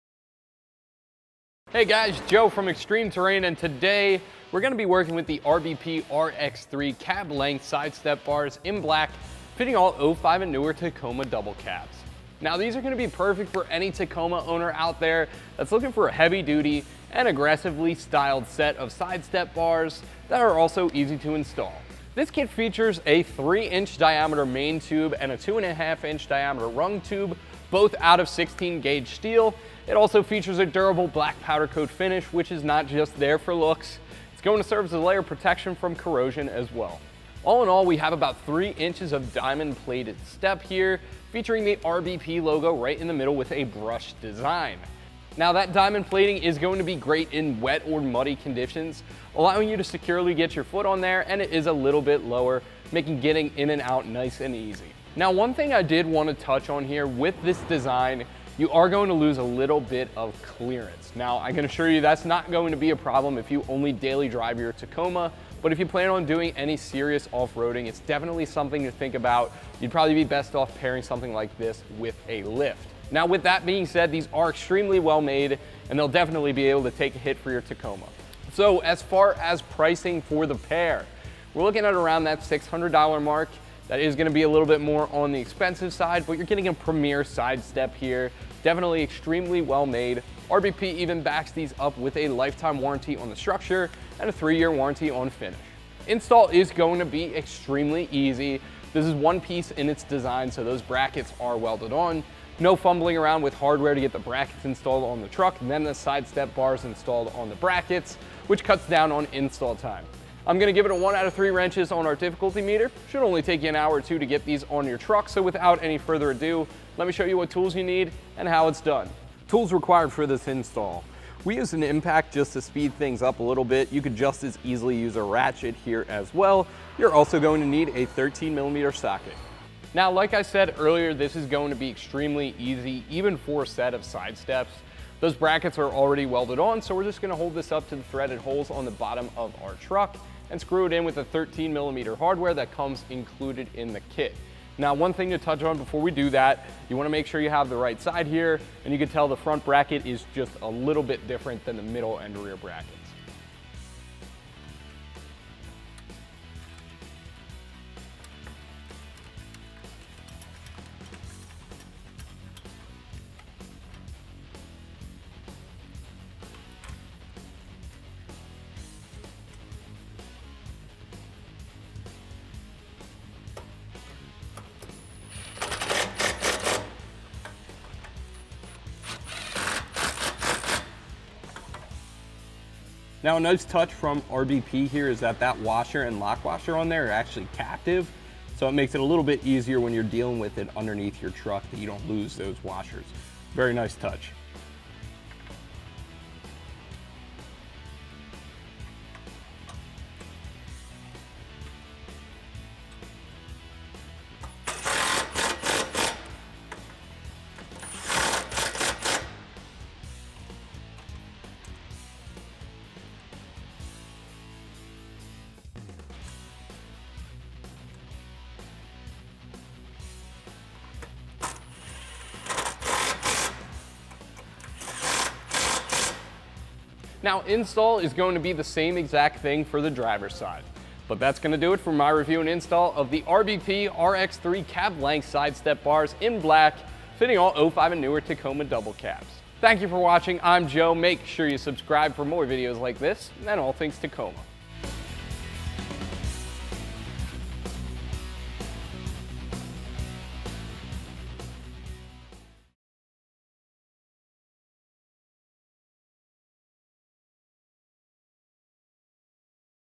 Tacoma RBP RX 3 Cab Length Side Step Bars - Black (2005-2019 Double Cab) Review & Install
Bolt on these RBP RX-3 Side Step Bars in Black to give your Tacoma some true off-road features. These side step bars are mounted close to the body, providing full ground clearance for your truck. They won’t snag or hinder your articulation as you conquer trails. Instead, they will provide more convenient and reliable entry and exit points in all weather conditions.

Item TT1957
MPN# RBP-1411B-RX3

ExtremeTerrain.com is your shop for the best in Toyota Tacoma aftermarket parts and accessories!